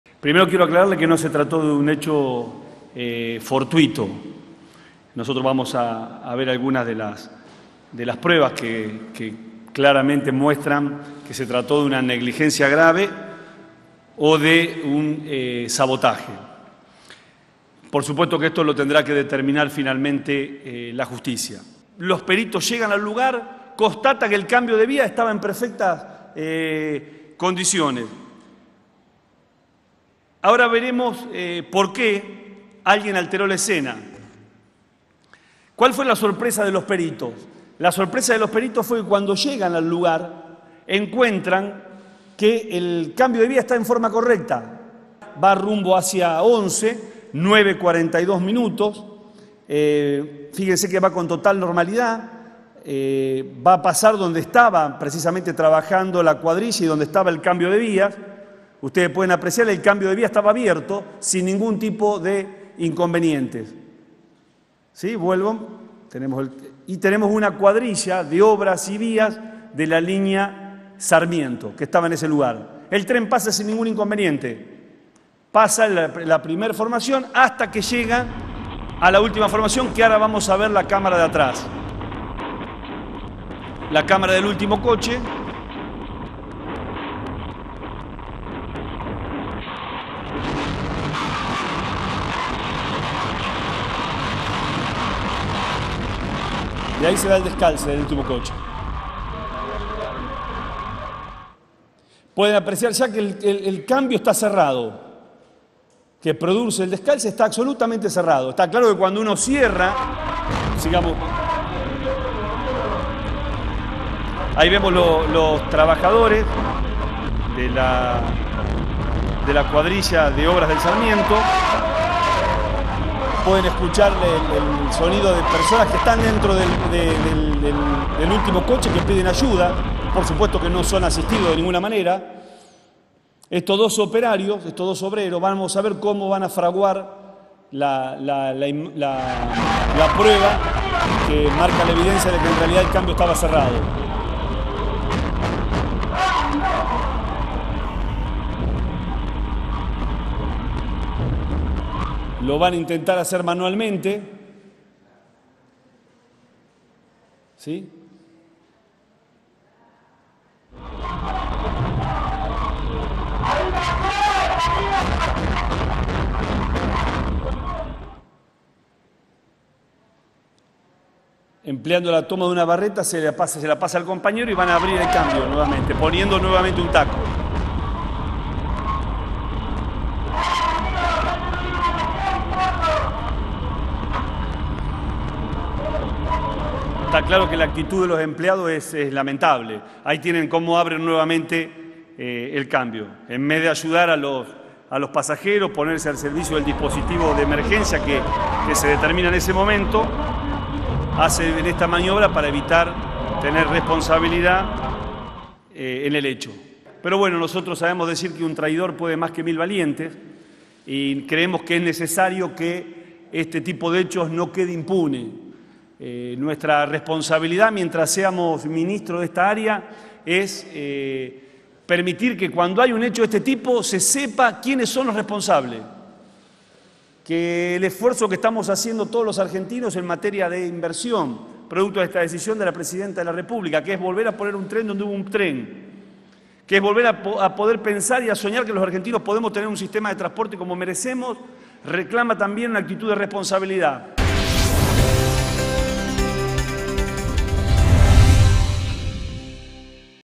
Randazzo denunció “negligencia o sabotaje” en el descalce del sarmiento del 30 de diciembre
El ministro del Interior y Transporte, Florencio Randazzo, denunció hoy que hubo “negligencia o sabotaje” por parte de dos trabajadores ferroviarios que habrían modificado el cambio de vía en momentos en que descalzó una formación del ferrocarril Sarmiento el último 30 de diciembre entre las estaciones de Haedo y Morón.
El funcionario exhibió una filmación tomada desde la cámara trasera del tren en el que se ve a obreros de una cuadrilla con una barreta que estarían modificando el cambio de vía en momentos en que pasaba el tren.
Explicó que el Gobierno presentó todas las pruebas ante el Juzgado Nro 3 de Morón, a cargo de Juan Pablo Salas, que investigará “si hubo negligencia o sabotaje”, pero mientras tanto la línea “inició el proceso de despido sobre los dos trabajadores”.